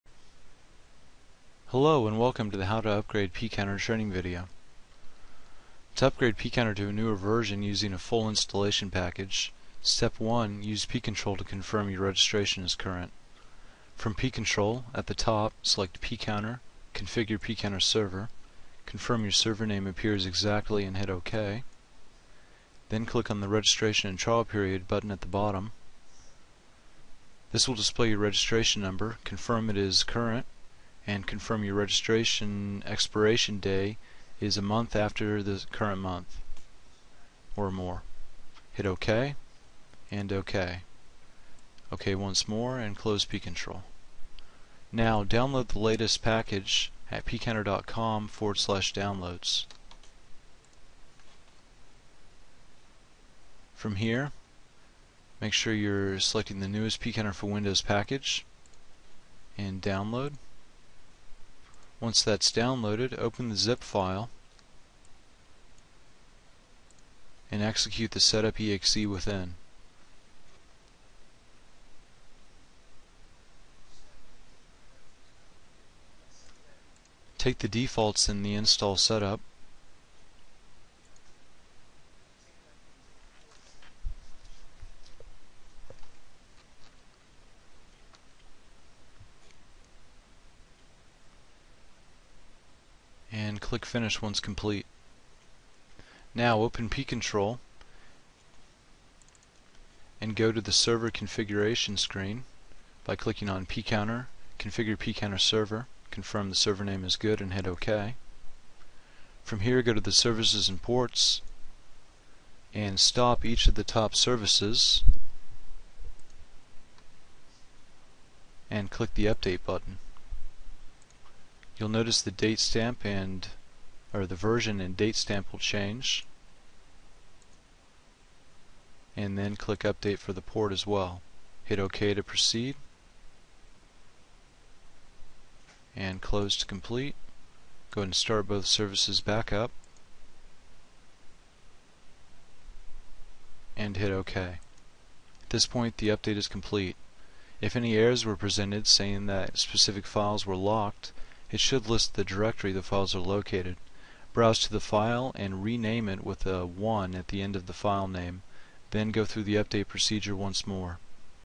How to upgrade Pcounter for Windows
This video provides step by step instructions for installing the latest Pcounter package.